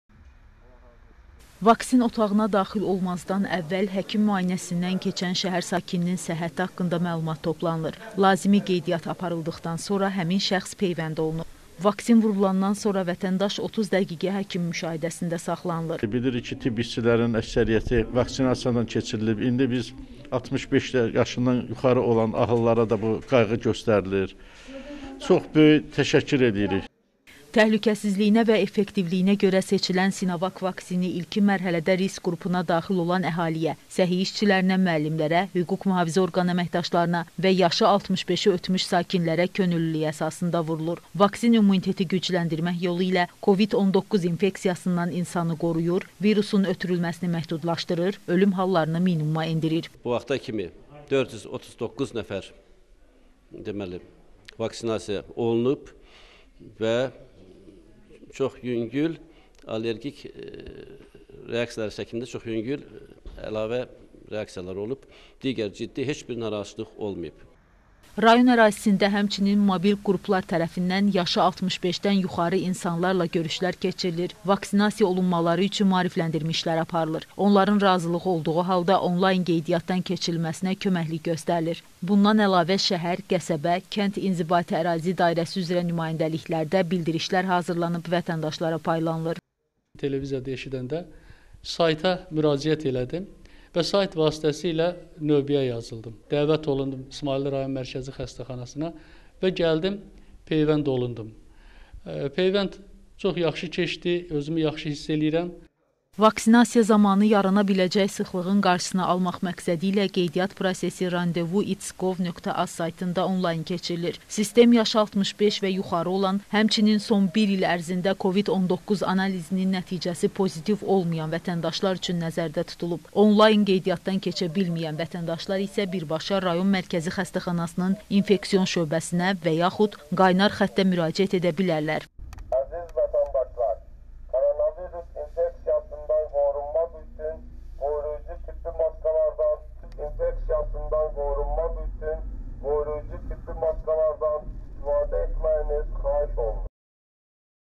İsmayillida yashi 65-den yuxari olanlar uchun vaksinasiya bashladi
İsmayıllıda da COVİD-19 infeksiyasına qarşı vaksinasiyanın növbəti mərhələsinə başlanılıb. Bu mərhələdə 65 yaşdan yuxarı vətəndaşlar peyvənd olunacaqlar. Xəstəxanada yaradılan peyvənd otaqlarında TƏBİB-in təlimatlarına uyğun zəruri şərait yaradılıb.
 Vaksin otağına​ daxil olmazdan əvvəl həkim müayinəsindən keçən şəhər sakininin səhhəti haqqında məlumat toplanılır. Lazımi qeydiyyat aparıldıqdan sonra həmin şəxs peyvənd olunur.  Vaksin vurulandan sonra vətəndaş 30 dəqiqə həkim müşahidəsində saxlanılır. Təhlükəsizliyinə və effektivliyinə görə seçilən “Sinovac” vaksini ilkin mərhələdə risk qrupuna daxil olan əhaliyə - səhiyyə işçilərinə, müəllimlərə, hüquq-mühafizə orqanı əməkdaşlarına və yaşı 65-i ötmüş sakinlərə könüllülük əsasında vurulur. Vaksin immuniteti gücləndirmək yolu ilə COVID-19 infeksiyasından insanı qoruyur, virusun ötürülməsini məhdudlaşdırır, ölüm hallarını minimuma endirir. Rayon ərazisində həmçinin mobil qruplar tərəfindən yaşı 65-dən yuxarı insanlarla görüşlər keçirilir, vaksinasiya olunmaları üçün maarifləndirilmə işləri aparılır. Onların razılığı olduğu halda onlayn qeydiyyatdan keçirilməsinə köməklik göstərilir. Bundan əlavə, şəhər, qəsəbə, kənd inzibati ərazi dairəsi üzrə nümayəndəliklərdə bildirişlər hazırlanıb vətəndaşlara paylanılır.  Vaksinasiya zamanı yarana biləcək sıxlığın qarşısını almaq məqsədilə qeydiyyat prosesi randevu.its.gov.az saytında onlayn keçirilir. Sistem yaşı 65 və yuxarı olan, həmçinin son bir il ərzində COVID-19 analizinin nəticəsi pozitiv olmayan vətəndaşlar üçün nəzərdə tutulub. Onlayn qeydiyyatdan keçə bilməyən vətəndaşlar isə birbaşa rayon mərkəzi xəstəxanasının infeksion şöbəsinə və yaxud qaynar xəttə müraciət edə bilərlər.